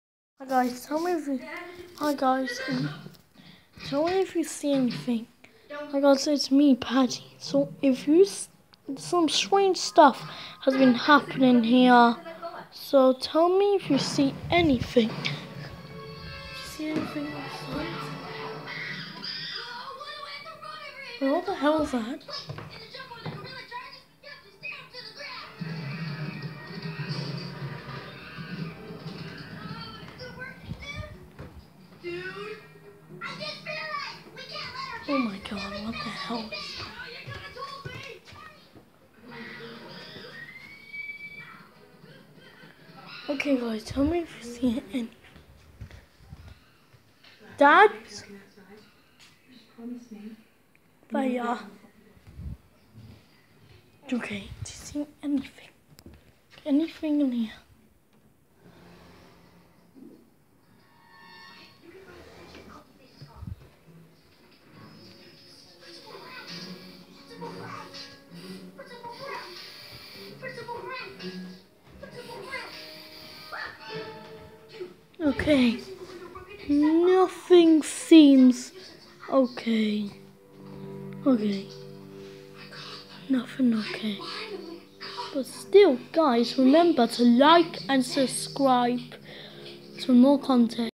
12.00 witching hour
Hi guys I just got back from holiday and everything's been a bit creepy so I comes to check and it's 12.00